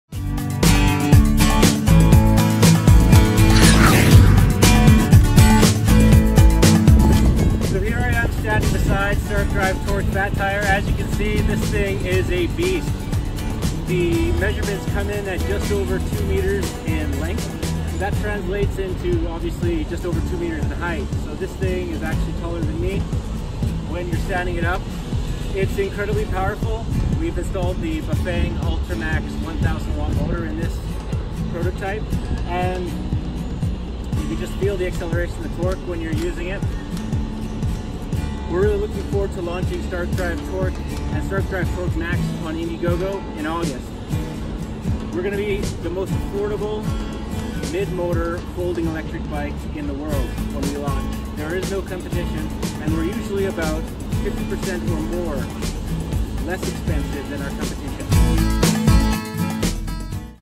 Stark Drive Torque Max Indiegogo Launch
🚀 August 20th 2019 12:00PM EST. Stark Drive Torque comes in two models standard tires and fat tire for the ultimate in off-road adventure 🚲mid-motor specifications. ⚡️Starting at under $1000. 🤩Up to 1000w of power and up to 160Nm Torque. 💪Double our nearest hub motor based competition at similar power. 🚀Indiegogo 2️⃣0️⃣1️⃣9️⃣No kidding mate!